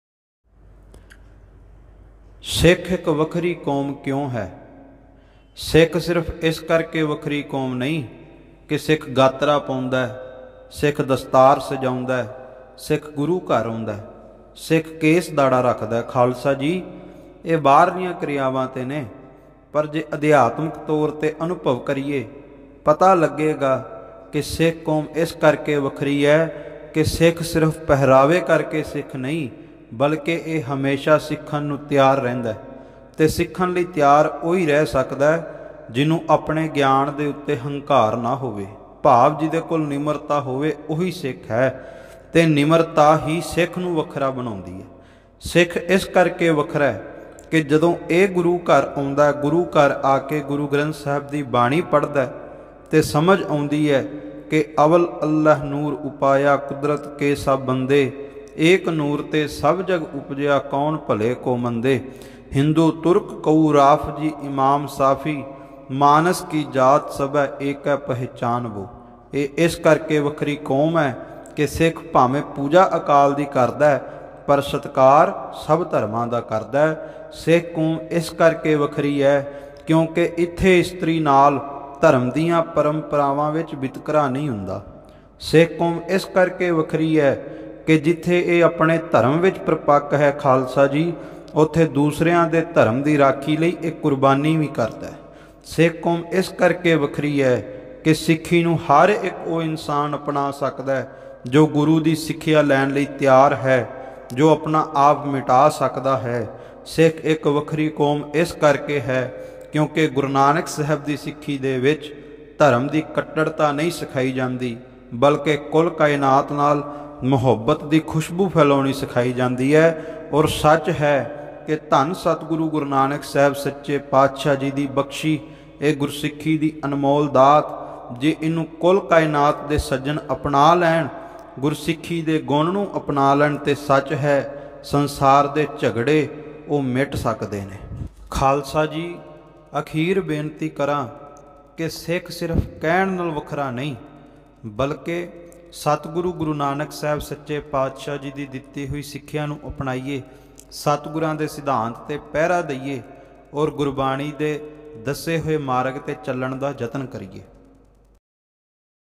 Sikh Vakhri Kaum Kyu Hai | ਸਿੱਖ ਵੱਖਰੀ ਕੌਮ ਕਿਉਂ ਹੈ | Gurbani Vichar | Bhai Sukhjeet Singh Ji Kanhaiya
| Sikh ik vakhri kaum kyu ha ? |

| vichar | bhai sukhjeet singh ji kanhaiya |
| mukh parcharak budha dal panjva takhat |